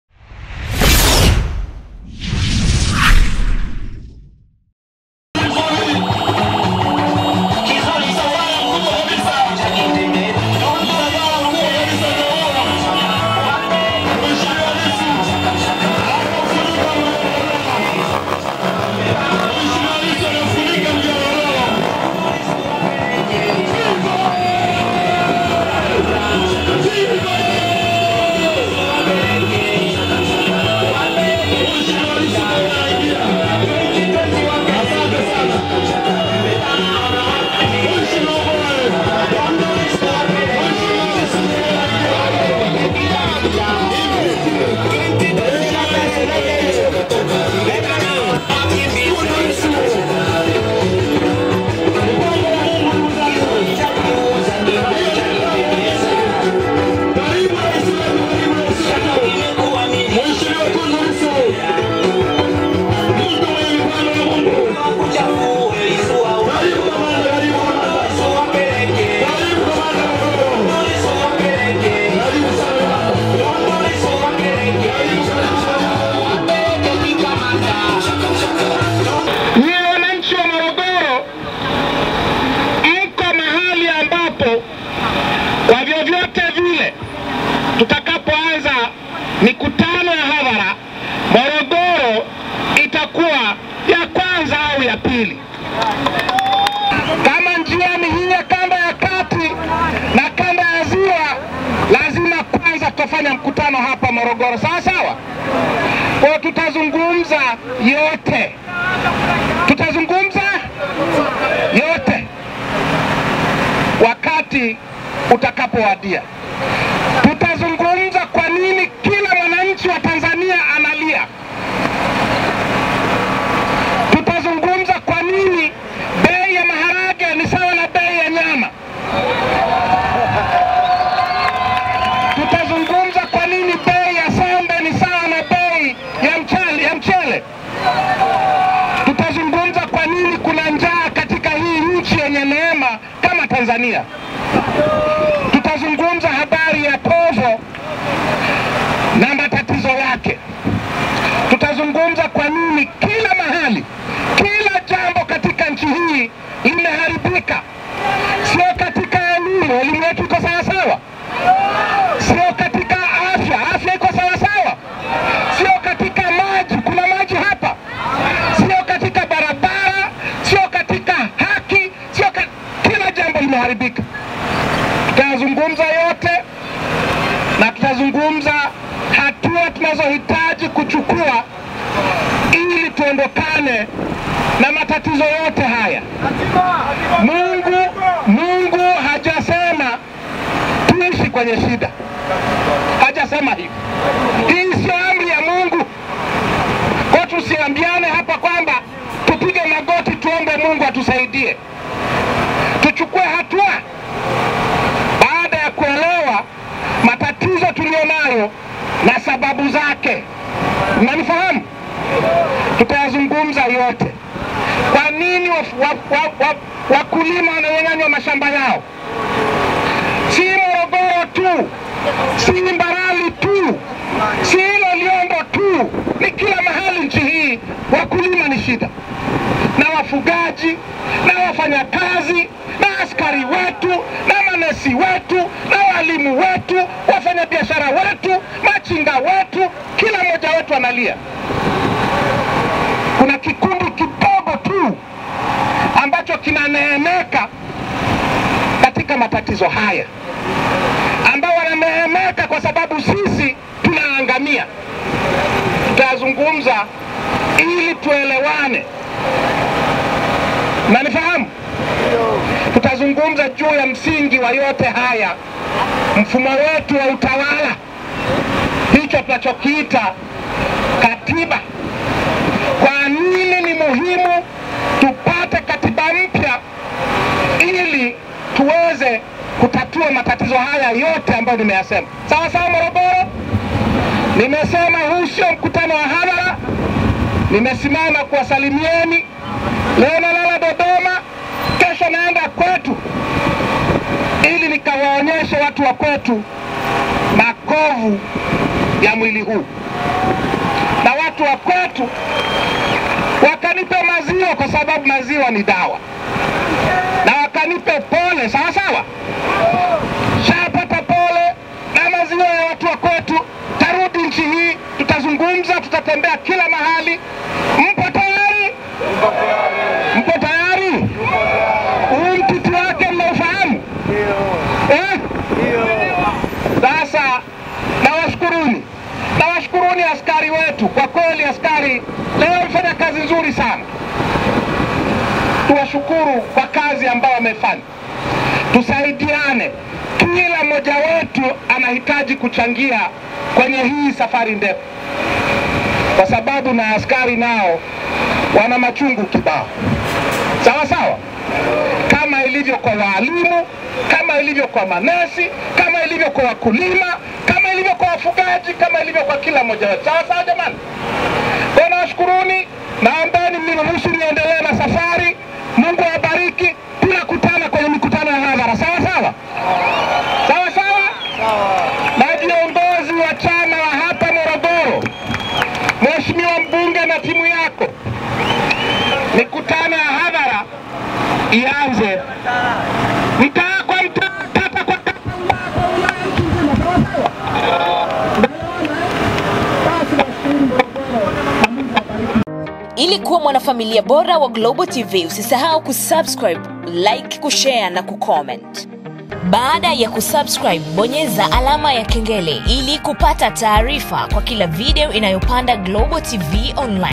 USIPIME! MSAFARA wa TUNDU LISSU MOROGORO, WAFUNGA BARABARA, ASIMAMA KUZUNGUMZA na WANANCHI...
Makamu mwenyekiti wa chama cha demokrasia na maendeleo CHADEMA Antpus  Tundu Lissu amezungumza na wakazi wa mkoa wa Morogoro katika safari yake ya akielea nyumbani  kwao  mkoani Singida

Akizungumza na mamia ya watu ambao wamejitokeza kumshuhudia, amewasihi kuendelea kuwa watulivu na pindi mikutano ya hadhara ya kisiasa itakapoanza watajadili kwa pamoja changamoto zinazolalmikiwa na wananchi ikiwemo mfumuko wa bei nataka.